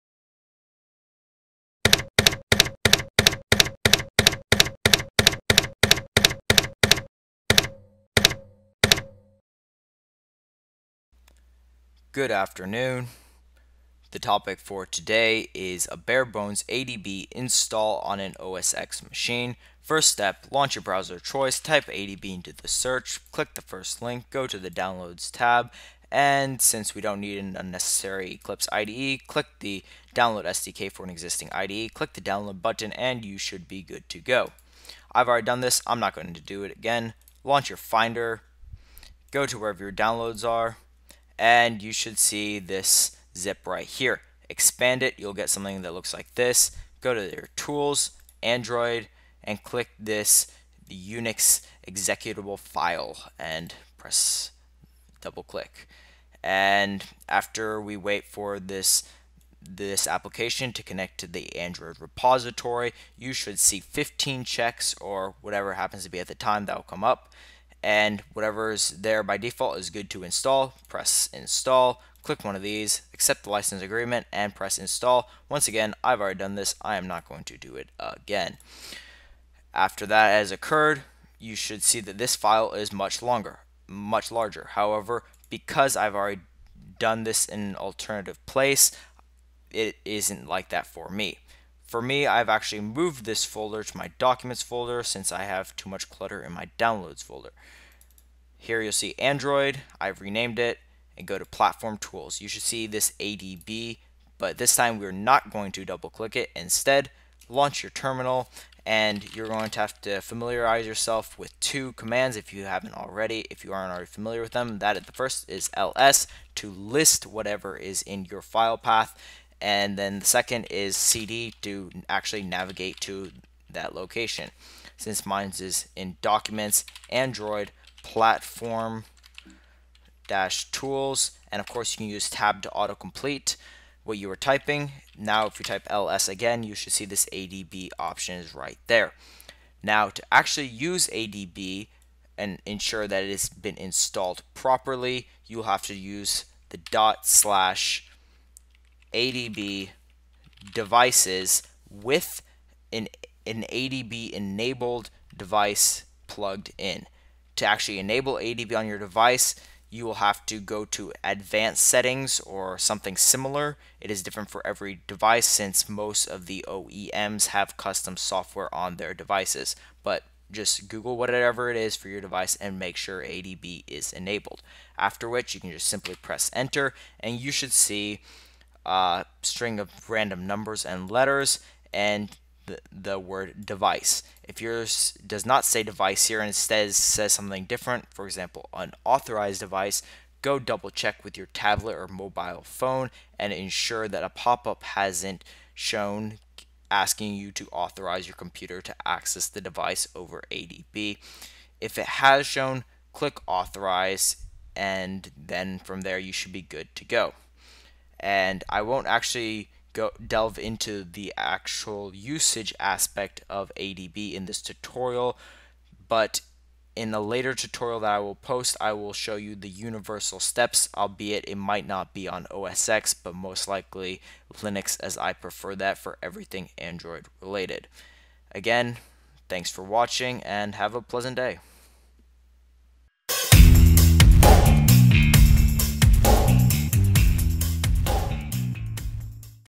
ADB How to: OSX installation
In this tutorial you will learn how to install ADB on an OSX setup.